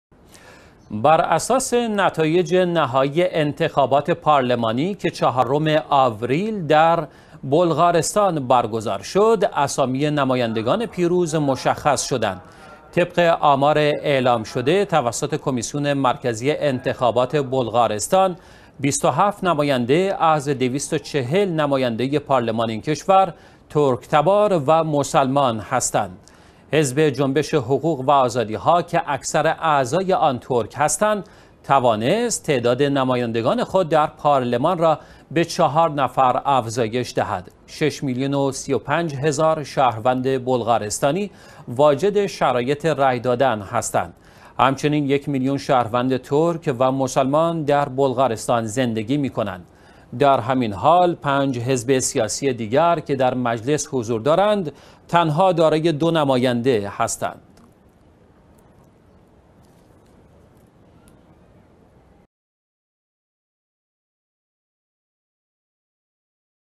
حضور پررنگ نمایندگان مسلمان در پارلمان بلغارستان
بر اساس نتایج نهایی انتخابات پارلمانی که چهارم آوریل در بلغارستان برگزار شد، اسامی نمایندگان پیروز مشخص شدند.
طبق آمار اعلام شده توسط کمیسیون مرکزی انتخابات بلغارستان، 27 نماینده از 240 نماینده پارلمان این کشور ترک ‌تبار و مسلمان هستند.
حزب جنبش حقوق و آزادی‌ها که اکثر اعضای آن ترک هستند، توانست تعداد نمایندگان خود در پارلمان را به چهار نفر افزایش دهد.
شش میلیون و 35 هزار شهروند بلغارستانی واجد شرایط رای‌‌ دادند هستند. همچنین یک میلیون شهروند ترک و مسلمان در بلغارستان زندگی می ‌کنند.
در همین حال پنج حزب سیاسی دیگر که در مجلس حضور دارند تنها دارای دو نماینده هستند.